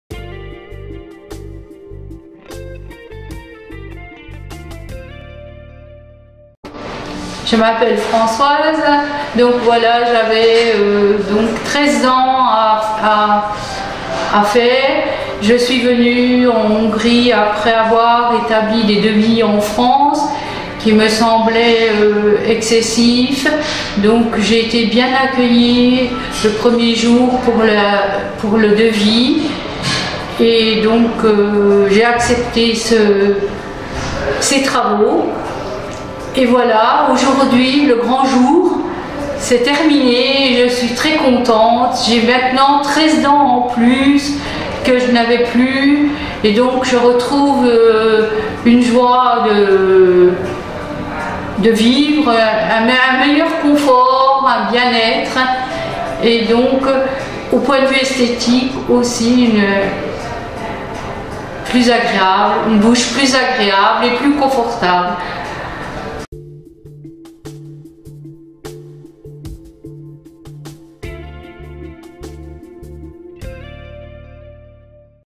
"13 dents en plus !" Témoignage d'implants dentaires Helvetic clinics Hongrie
Implants Dentaires. "Aujourd’hui le c’est le grand jour : c’est terminé et j’ai 13 DENTS EN PLUS que je n’avais plus...et je suis très contente! Le résultat est aussi esthétique, Helvetic clinics, une super clinique dentaire à Budapest".

Avis et témoignage d'un patient de Helvetic Clinics Budapest, Clinique dentaire et dentistes en Hongrie.

Helvetic clinics, clinique dentaire
12 Revay Ut, 1065 Budapest, Hongrie